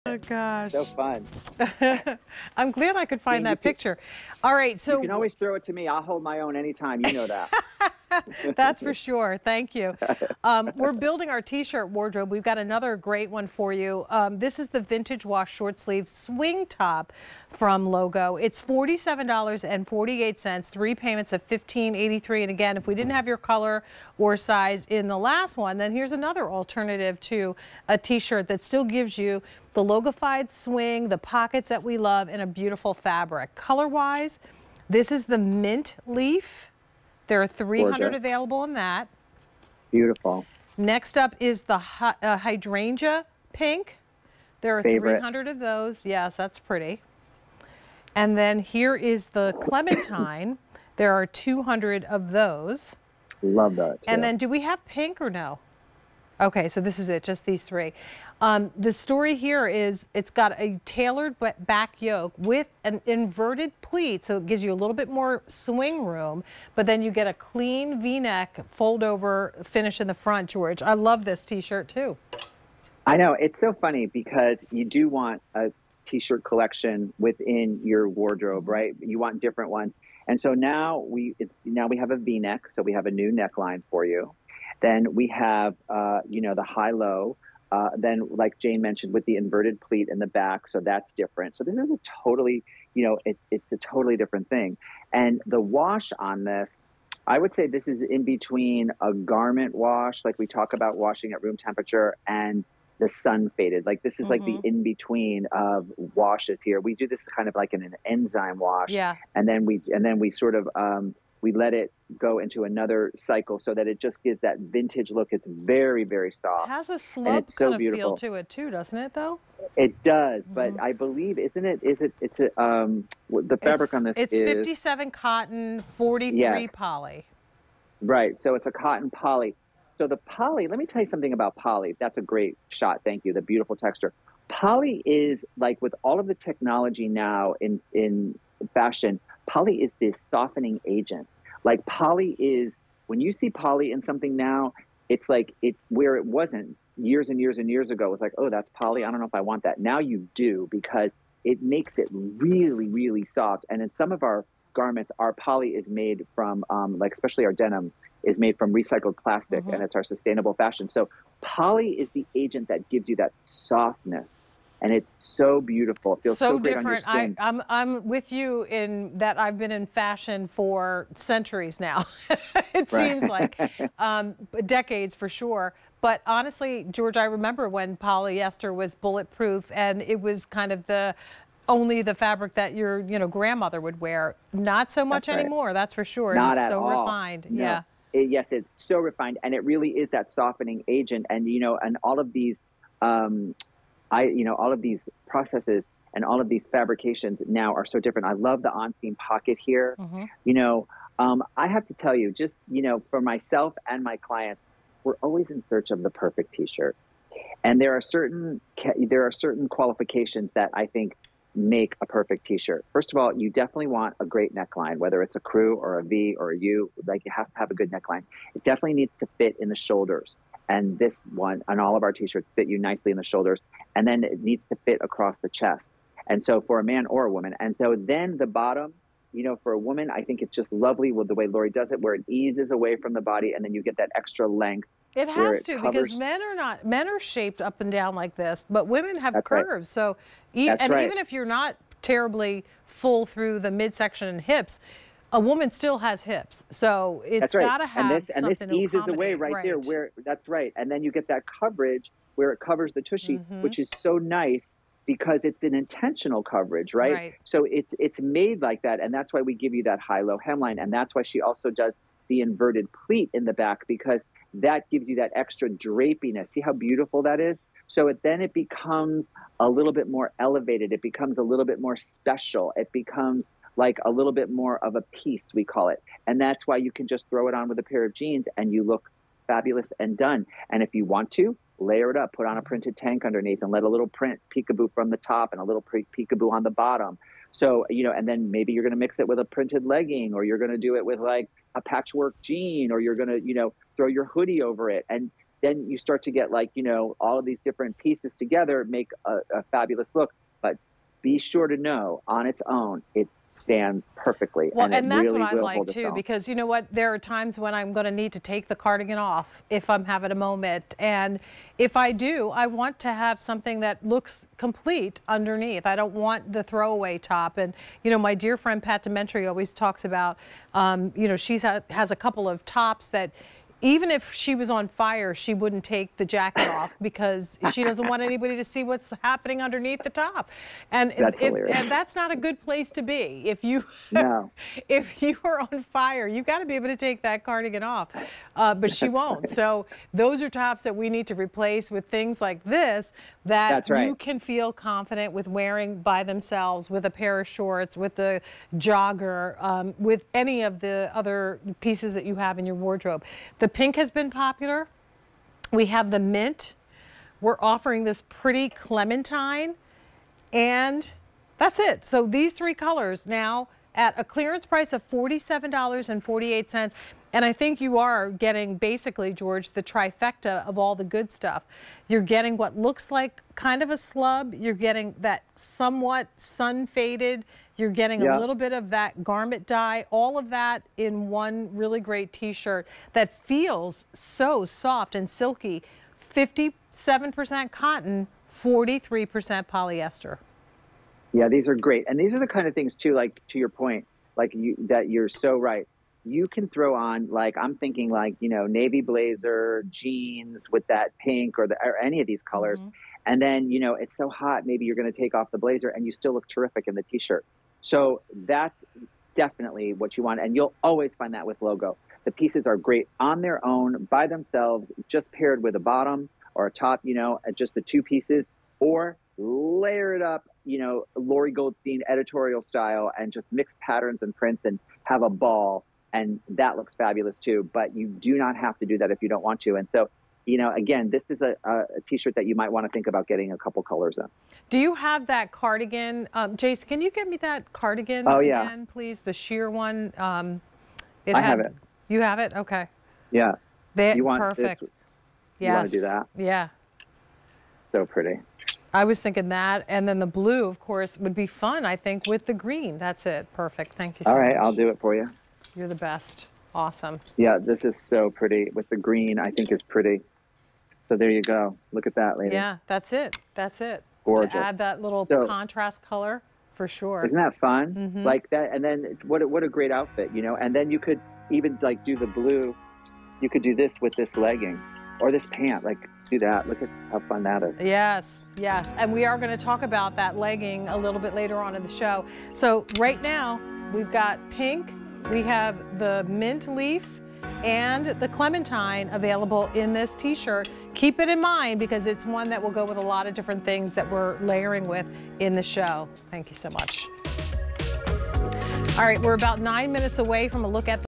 LOGO by Lori Goldstein Vintage Wash Short Sleeve Swing Top on QVC
LOGO by Lori Goldstein Vintage Wash Short Sleeve Swing Top

Top. This LOGO top features a v-neck, short sleeves, pleated back swing, and on-seam pockets. Semi-fitted: follows the lines of the body for added wearing ease Lengths:Missy Range:26.5"-28.625"; Plus Range:29"-32.5".   Content: 57% cotton 43% polyester_ Care: MWCGCWLC/ONCBWN/TDL/WIIN. COO:Imported.